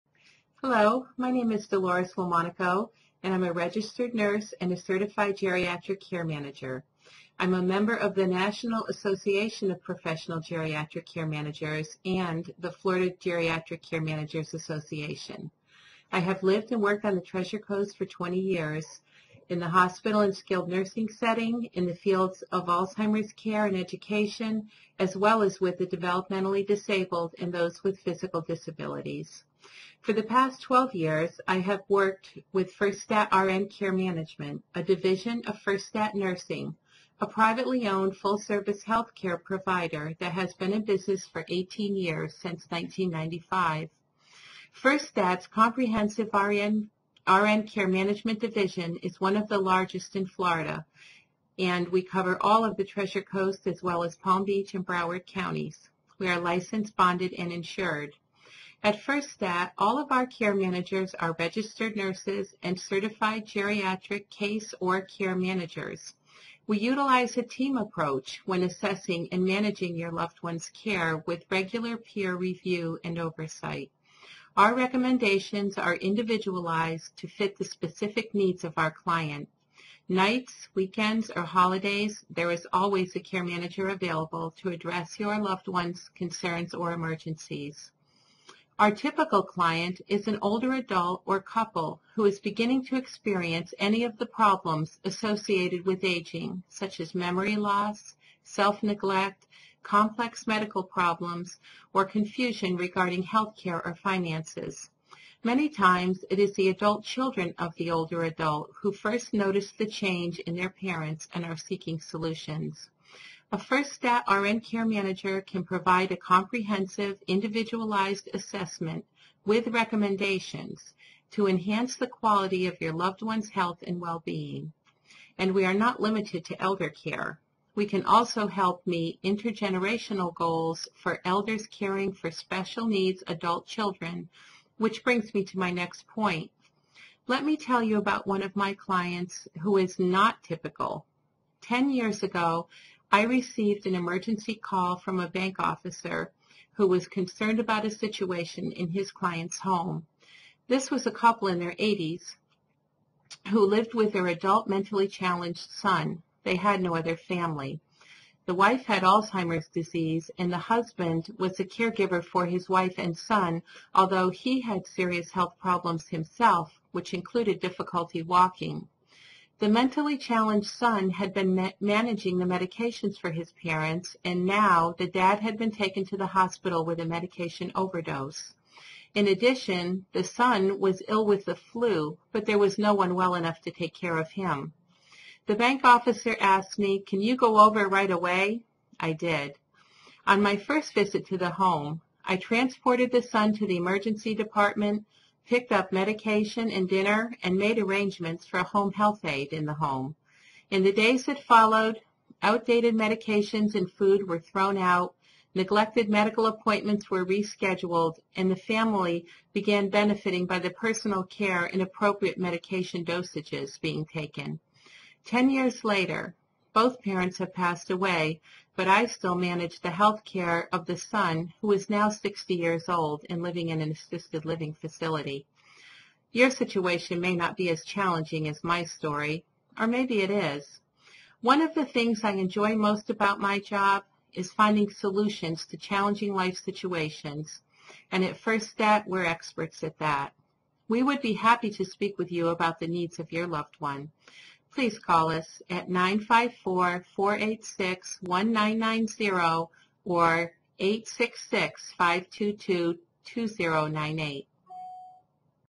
About Firstat RN Care Management Services -- Treasure Coast Geriatric Care Manager
Dolores LoMonaco of Firstat RN Care Management discuss services offered by Firstat RN Care Management.